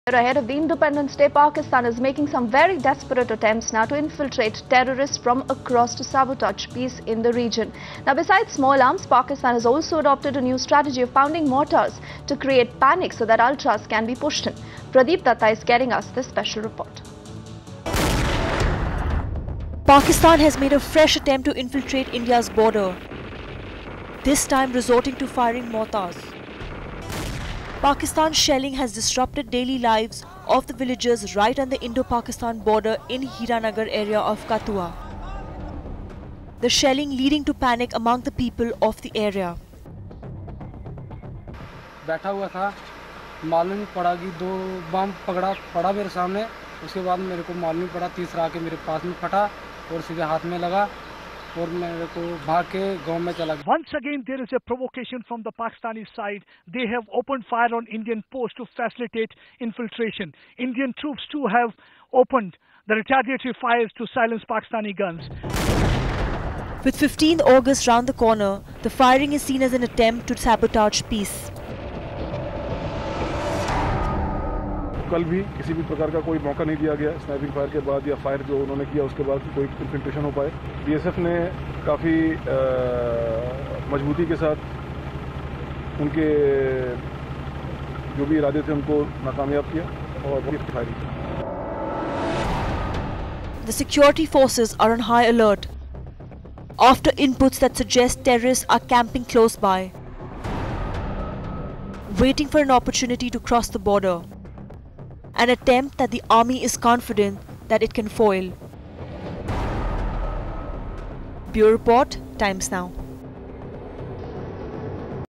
Pakistan's new strategy exposed
Is Nawaz Sharif pushing an anti-India campaign? Ahead of the crucial Indio-Pak meet in New York, TIMES NOW has learnt that Pakistan Prime Minister has ordered the formation of a Kashmir cell. That will make all efforts to reignite the K-debate. The move, has not just exposed Pakistan's double speak, It has also brought the focus on India's next strategic move after the fresh developments. With almost 12 ceasefire violations reported after Nawaz Sharif took over. The question now is, will India talk tough? Download the Times Now India's Election HQ app and get all the election info at one go. Social Media Links